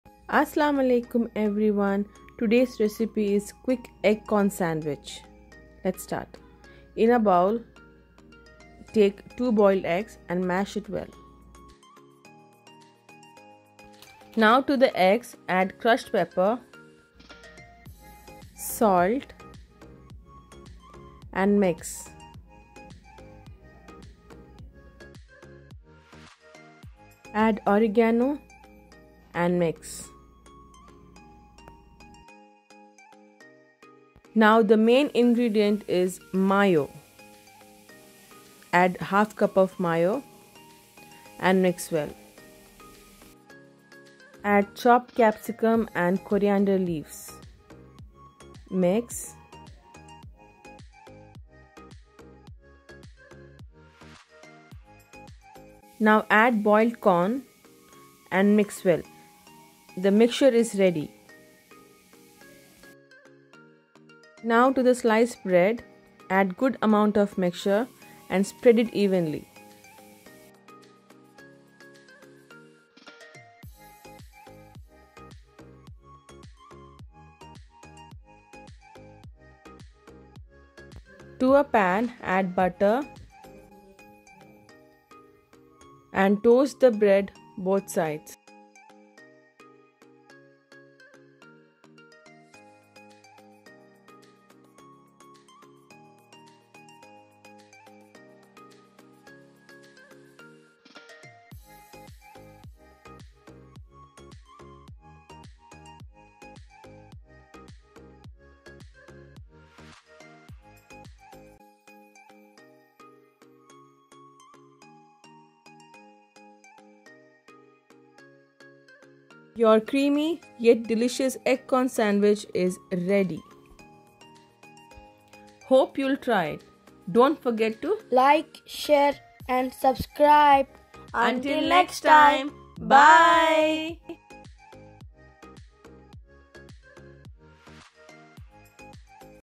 Egg Corn sandwich 🥪
Ingredients
● Boiled Egg -2
● Boiled Corn - 1/3cup
● capsicum -1/3cup
● Corriander leaves
● Crushed pepper
● Salt as per taste
● Plain mayonnaise - 1/2cup
● Oregano